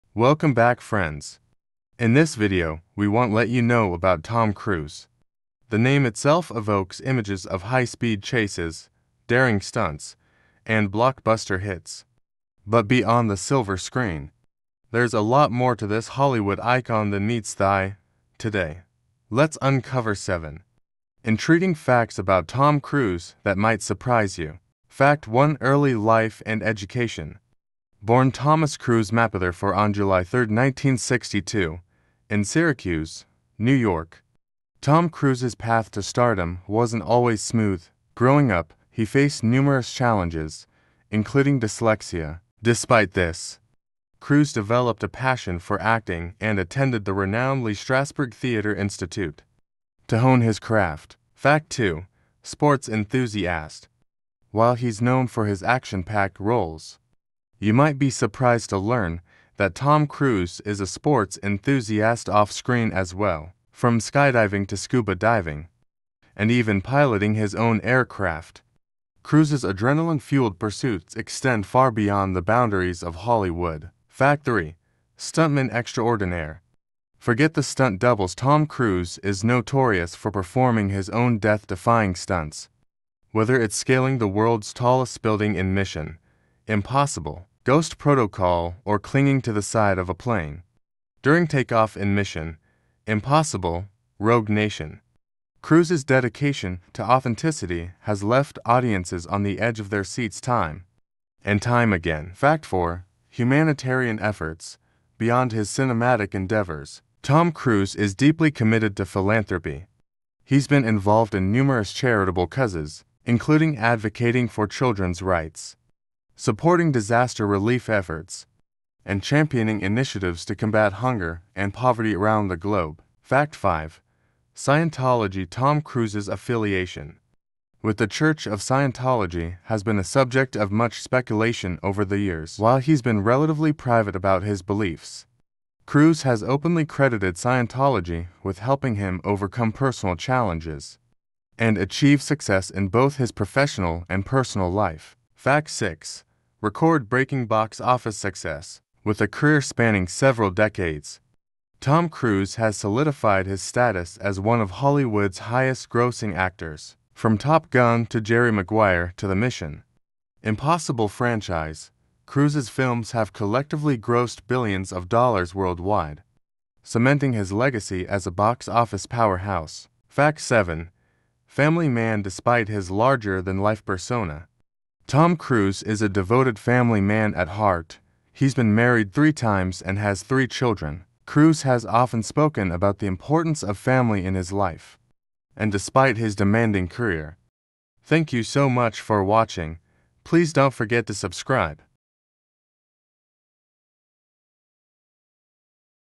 Tom cruise - 7 intriguing facts about Tom Cruise that might surprise you.
In this video, we delve deep into the life and career of one of Hollywood's most iconic figures: Tom Cruise. Known for his heart-pounding action sequences and charismatic performances, Cruise's journey from humble beginnings to international superstardom is nothing short of remarkable.

Join us as we uncover seven fascinating facts about Tom Cruise that might surprise you. From his early struggles with dyslexia to his fearless approach to performing his own stunts, Cruise's story is one of perseverance, passion, and unwavering dedication to his craft.

But there's more to Cruise than meets the eye. Beyond the glitz and glamour of Hollywood, he's a sports enthusiast, a dedicated philanthropist, and a devoted family man. Through his involvement in charitable causes and his commitment to his loved ones, Cruise demonstrates that true success extends far beyond the silver screen.

Whether you're a longtime fan or just getting to know the man behind the movies, this video offers a glimpse into the fascinating world of Tom Cruise. So sit back, relax, and prepare to be captivated by the extraordinary life of one of cinema's most enduring legends.

[Include links to your social media accounts and other relevant videos]